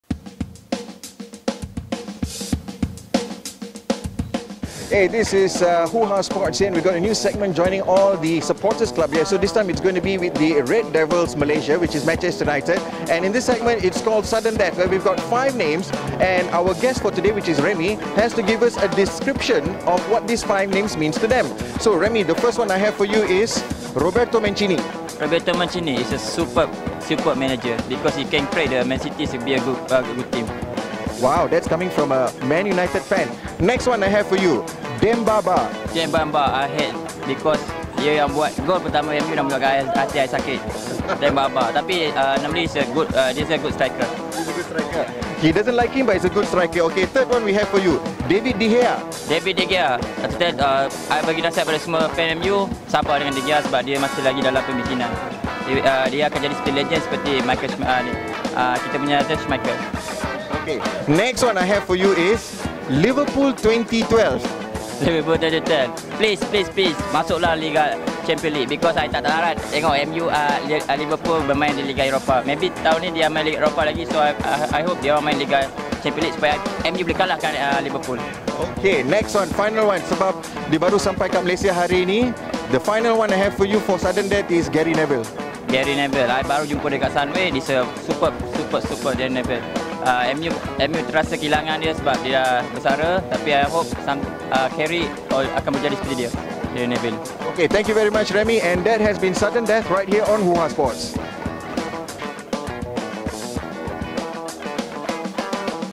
What Man. United Fans Think of Mancini, Ba, Liverpool 2012 - Hoohaa Sports
We begin 2012 with a new segment on Hoohaa Sports, called "Sudden Death". 5 names are given and they have to describe what they mean to a typical fan. We start with a Manchester United fan from Malaysia Red Devils, where 2 out of the 5, includes Mancini and Liverpool.